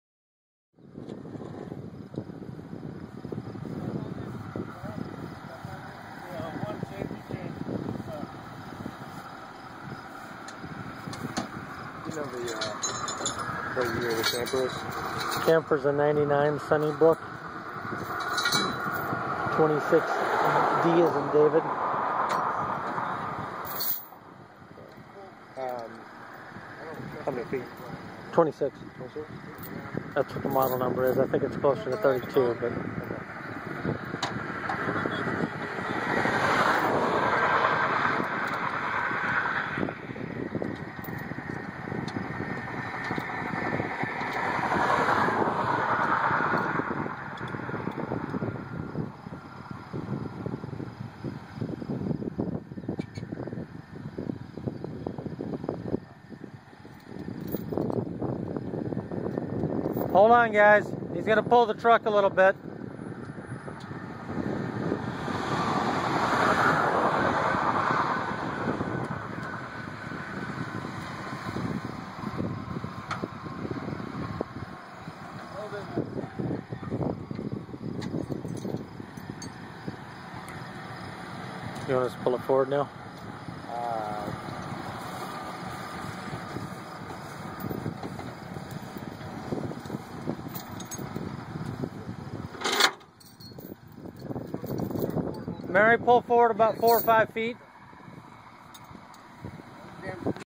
1st day of Vacation
This is what happens when you do not listen to your body when it tells you it is time to pull off and get some sleep!!!  Please do not follow in our footsteps and repeat this accident!!  Stay mentally and physically prepared for road trips.  This was day one of what we hope to still remain a 5 week family vacation and hopefully be able to add some MORE MEMORABLE events to this trip than the 1st days events!!